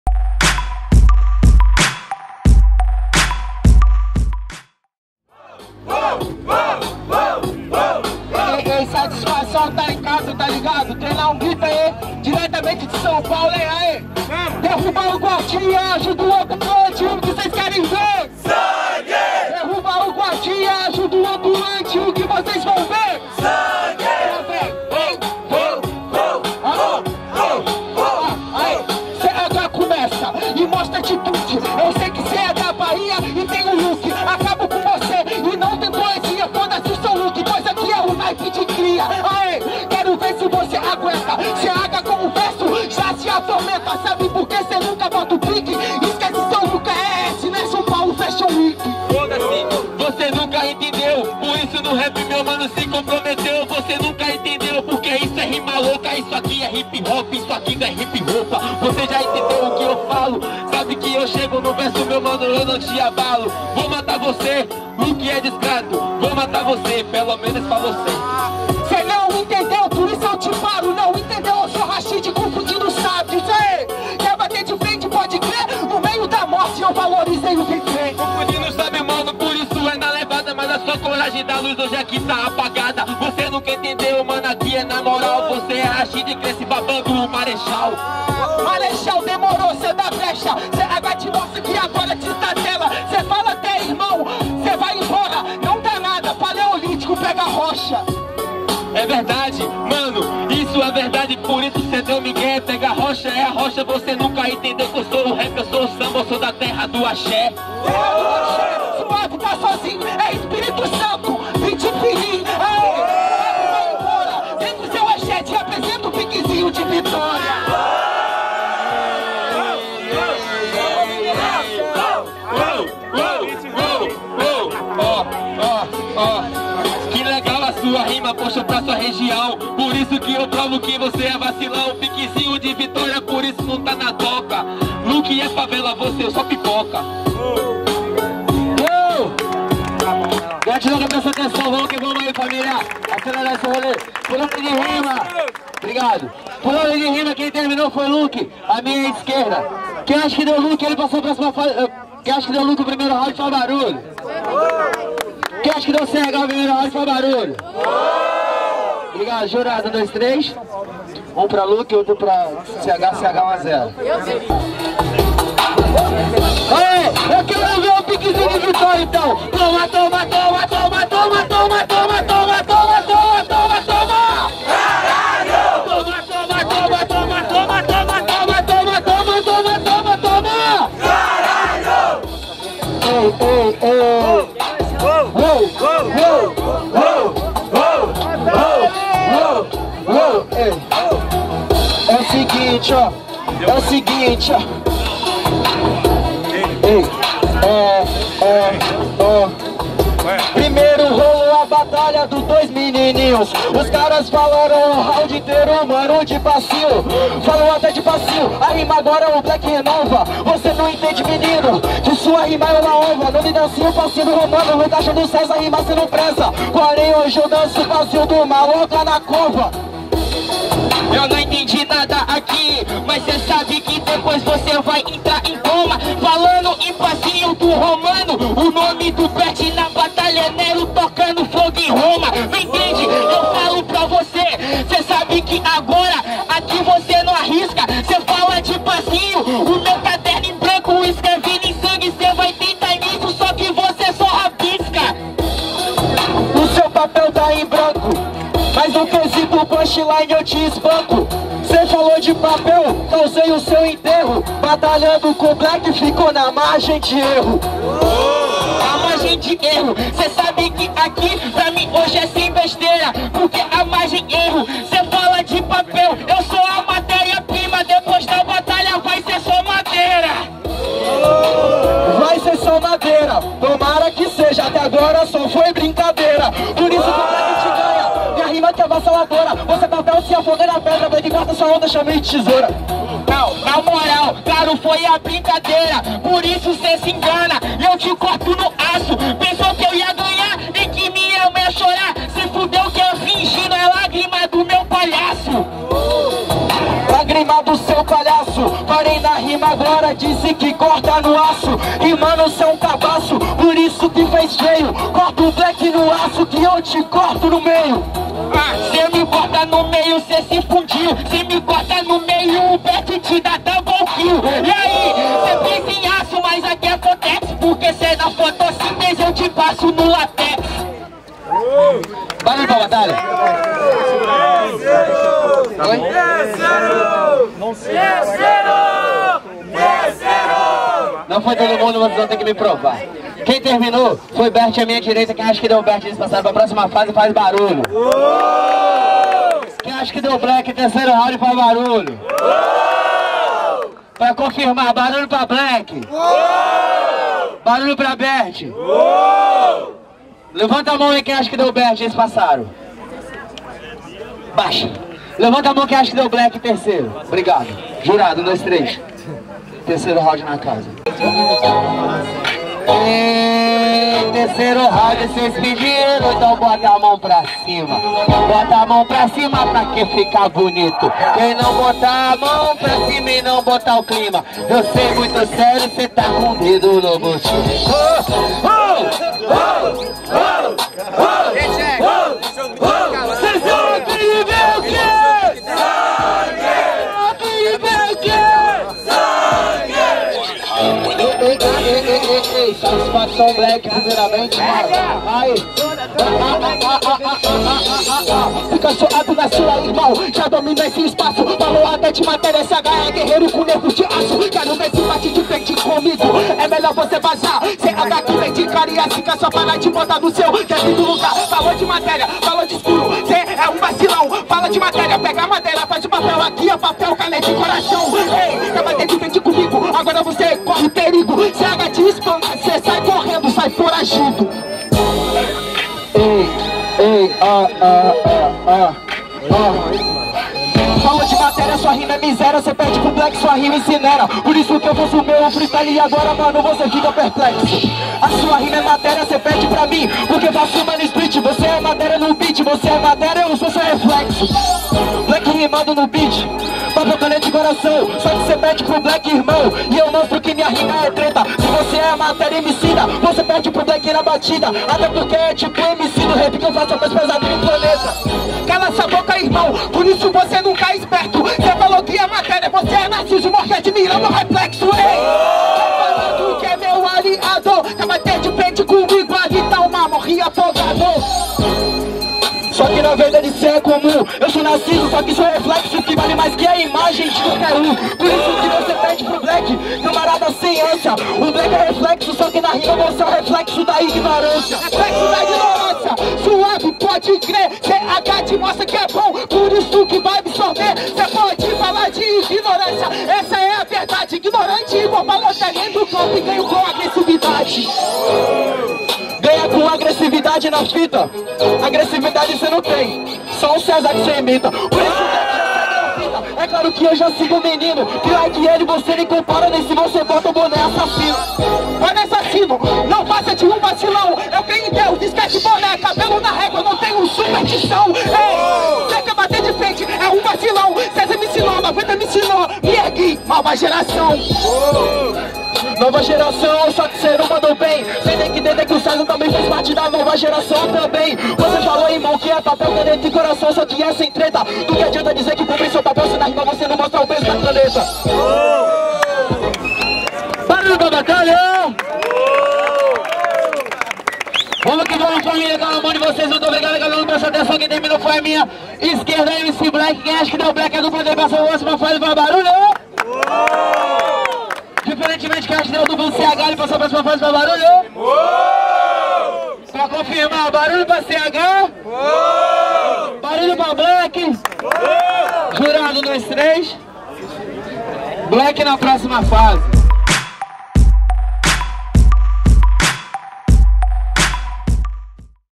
ESPECIAL BATALHA DO ATLÂNTICA - BLACK E LUKE X CH E BERT - 2ª FASE
Mar de Monstros (Canal do Dimas) apresenta Batalha do Atlântica, edição de dupla com participação dos representantes da Bahia.

AJUDE O MAR DE MONSTROS A REALIZAR EDIÇÕES INTER-ESTADUAIS, FAÇA SUA DOAÇÃO NO LINK ABAIXO!!!